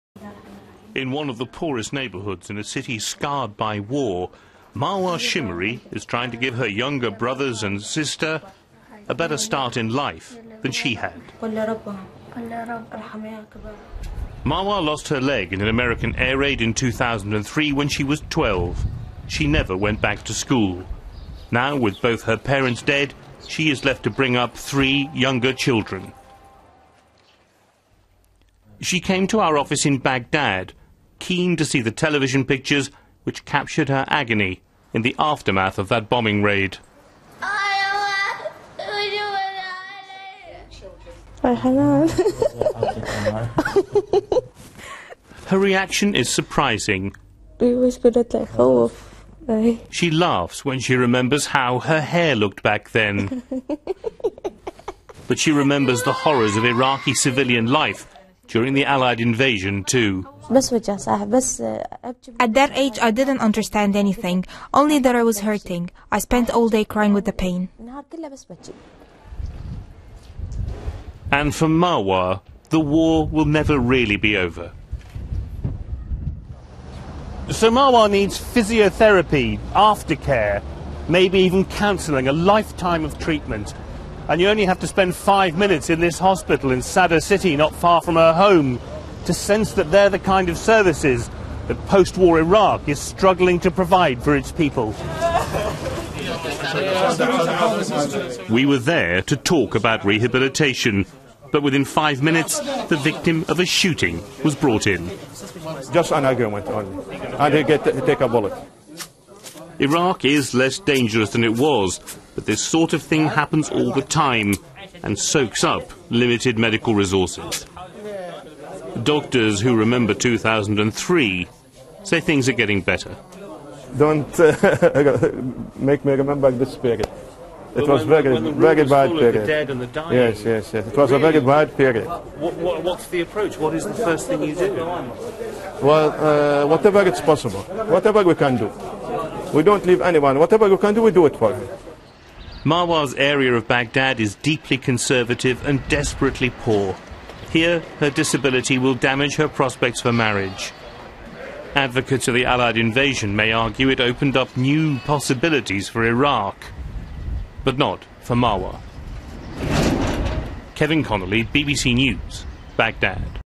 Marwa&#39;s story- Ten years since the bomb fell
As the US military fought their way into Baghdad ten years ago, the life of one Iraqi girl was changed forever when she was gravely injured in an air raid. M...